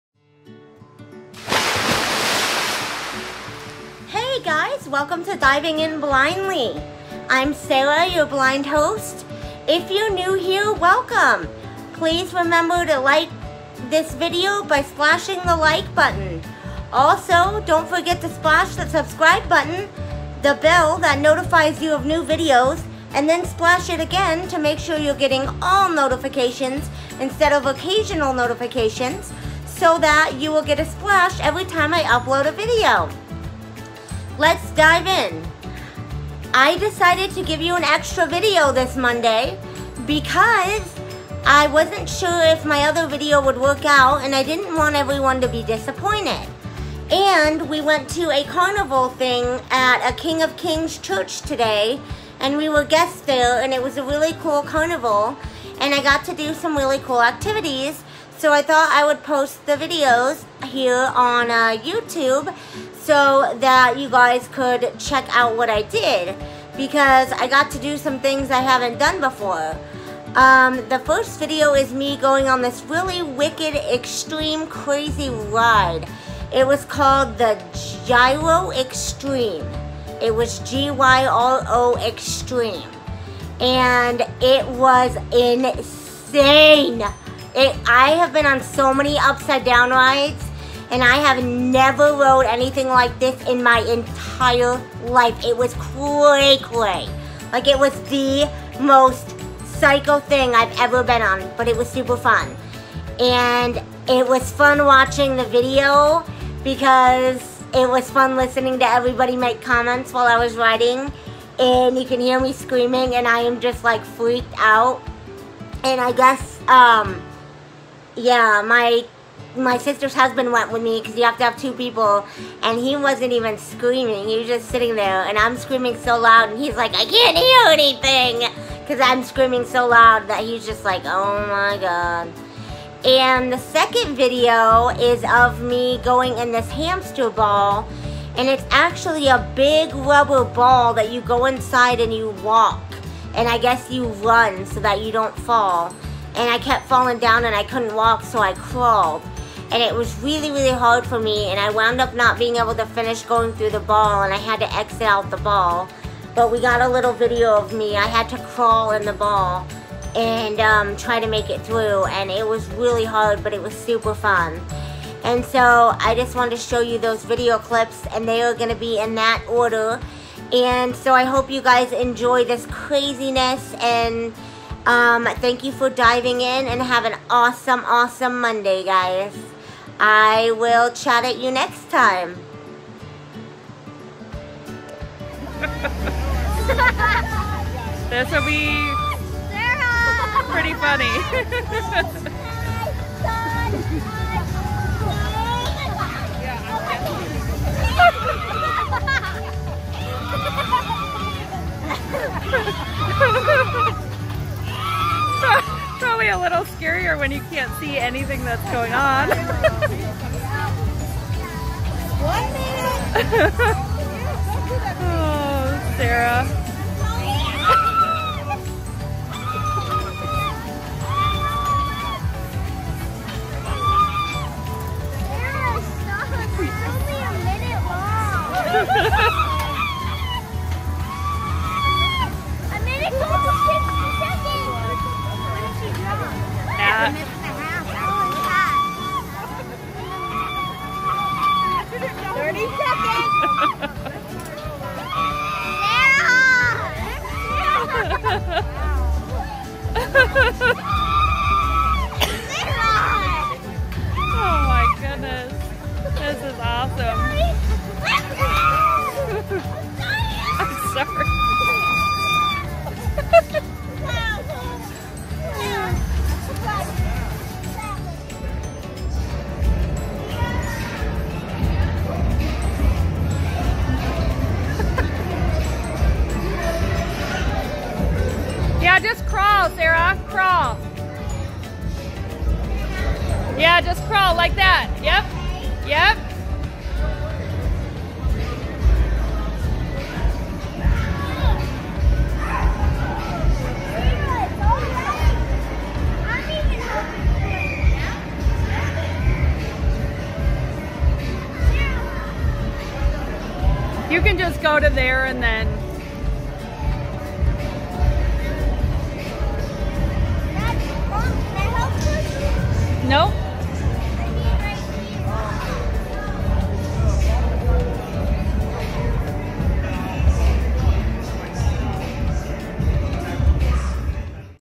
Craziest upside down ride I’ve ever been on!
We went to a little church carnival thing and I went on an upside down ride and I also went in a big inflatable ball and I thought I would give you a glimpse inside! :-)